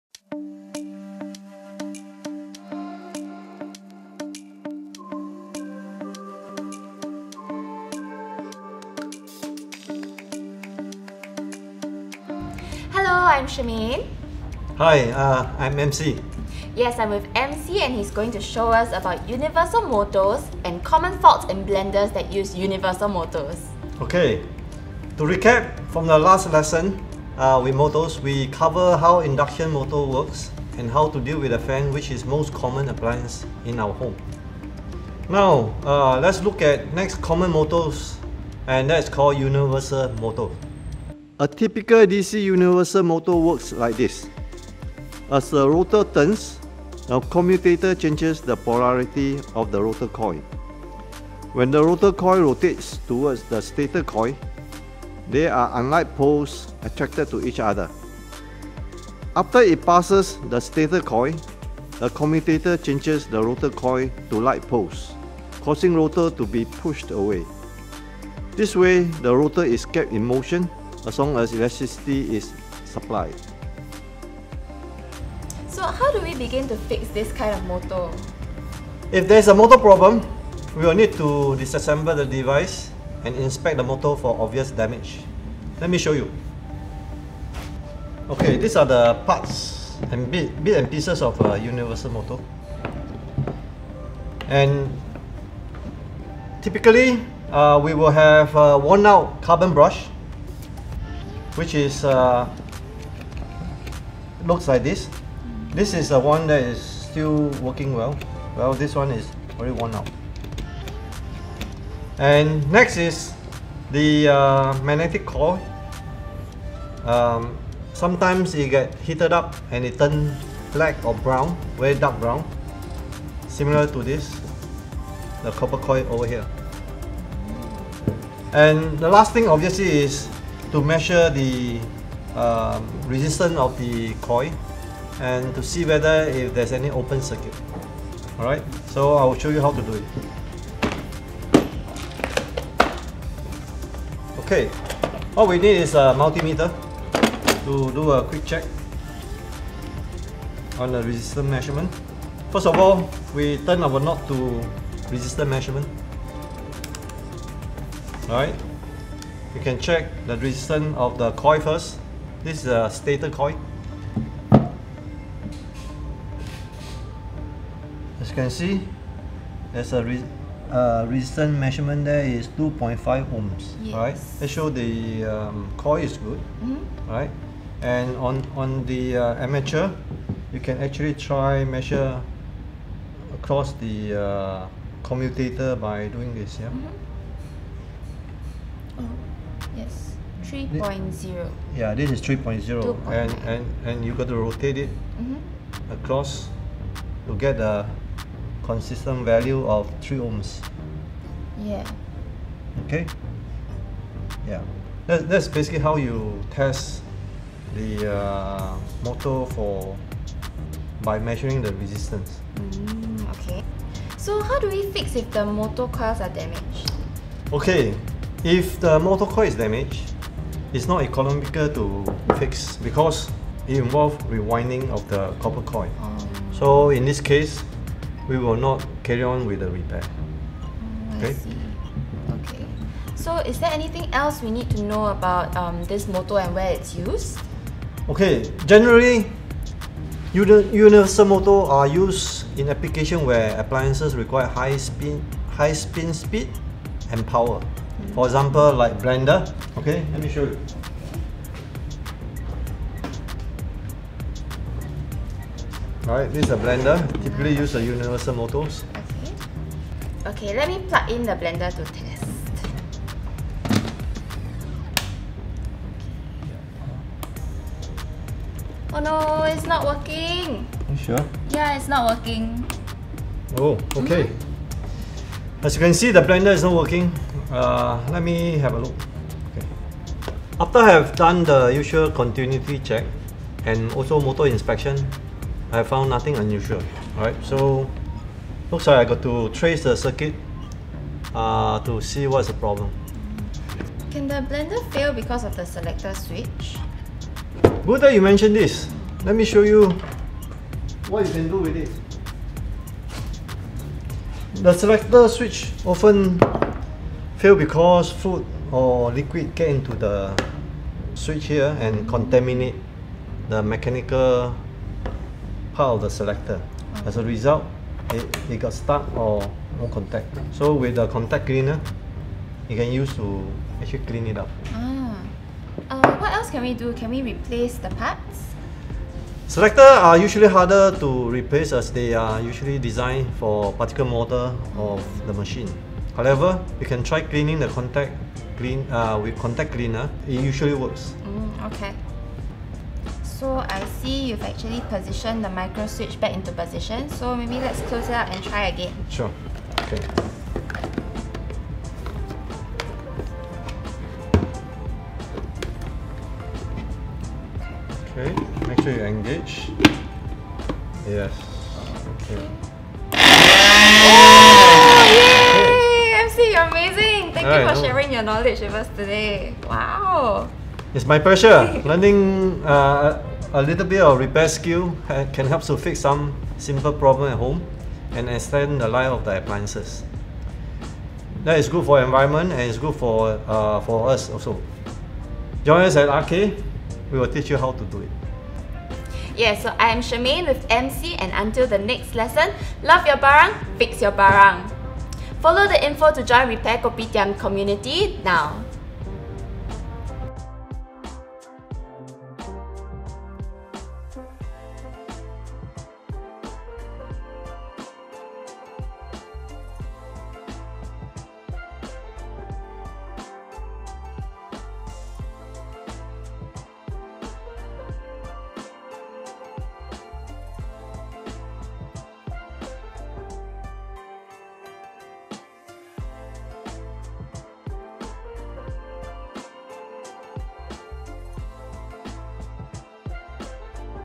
RKS EP5 Basic Motor Appliance Repair
Charmaine and MC team up again to cover the universal motor and how to fix a typical blender that uses this motor type.w

take you through the basics of motor appliance repair. This episode, on a very basic level, Repair Kopitiam Singapore coach Ming Ching will introduce the induction motor and how it is used in appliances. He will also show you how you can fix these appliances with induction motors.

Content covers:
1. How the universal motor works
2. How to check by visual inspection and resistive checking of the coils to verify the condition of the motor.
3. The carbon brush is shown and how to inspect the possible wear.
4. repositioning/replacing a faulty microswitch can easily fix some alignment problems
5. Common components selector switches and microswitches.
6. How to clean up a faulty selector